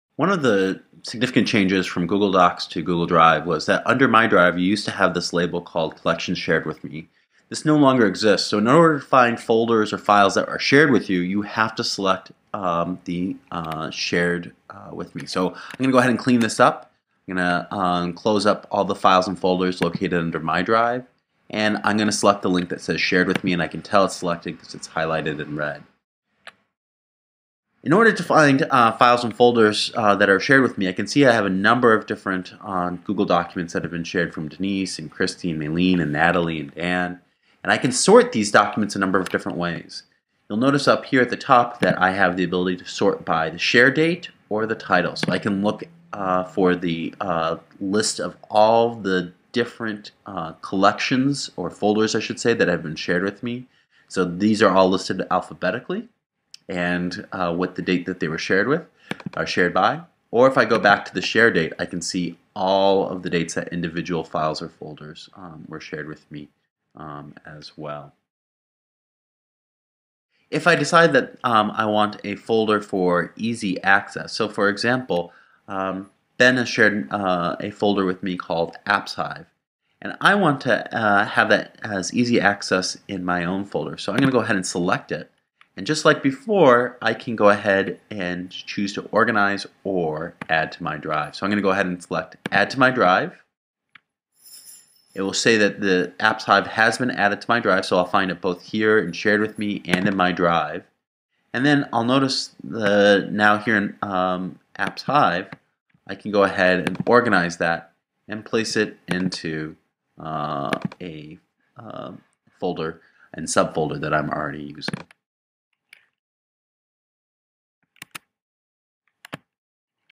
Finding and Organizing Folders or Documents Shared with Me in Google Drive
Key Points:
Finding files "Shared with me"
Sorting files "Shared with me" by date, by title
Adding folders from "Shared with me" into "My Drive"

Reference: Molly Schroeder's Tech for Teachers - Google Drive (Permission Granted by Creator).